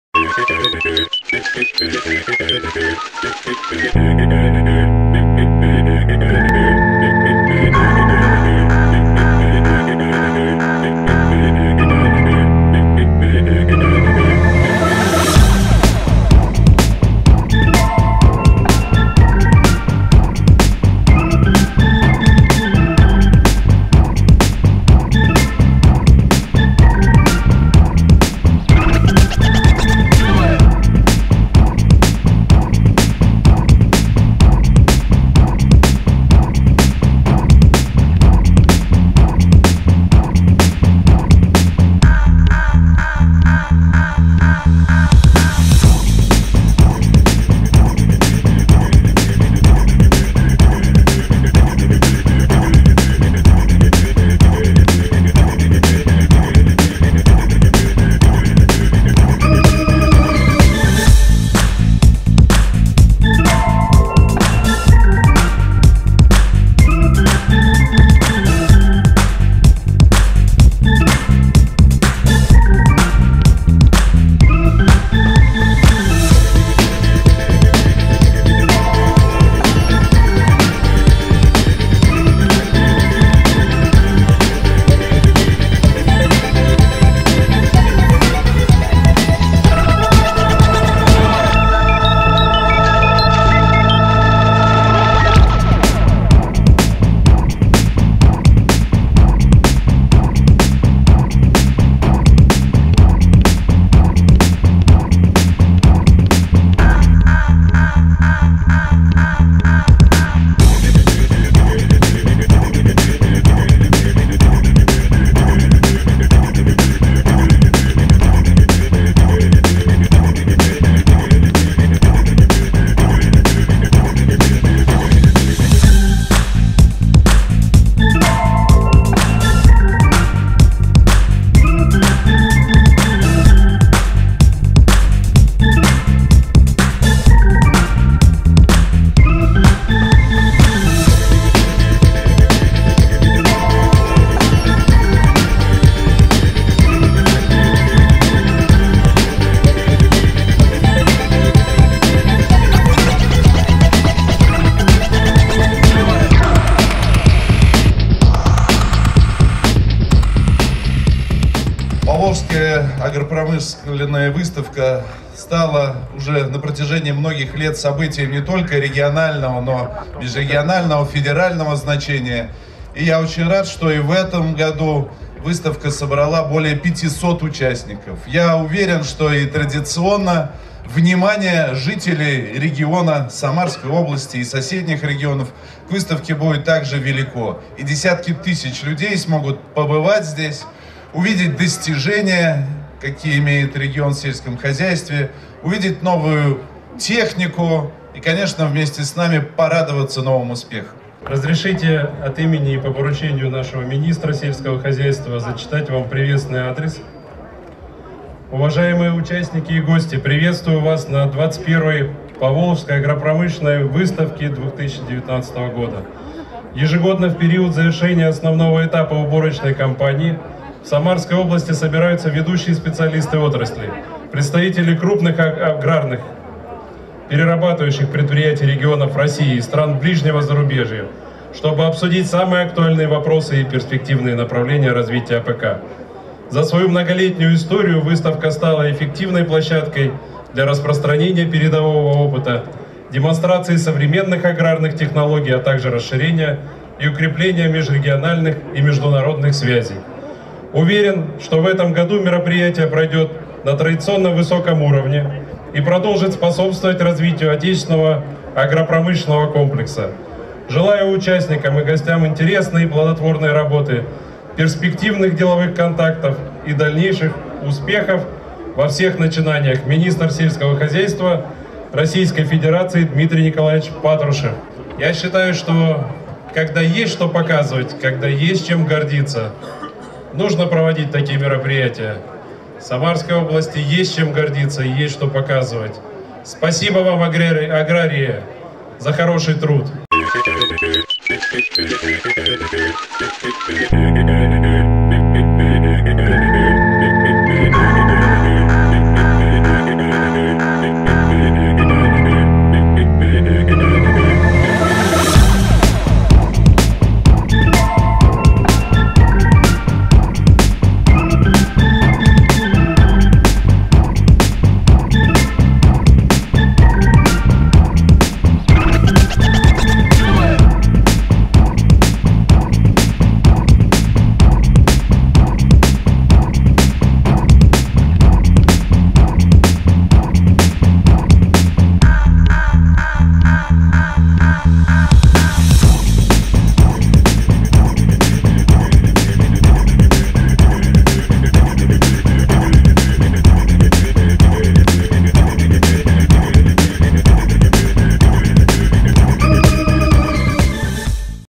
XXI Поволжская агропромышленная выставка
Выставка – 2019. Самарский аграрный университет вносит свой весомый вклад в развитие АПК региона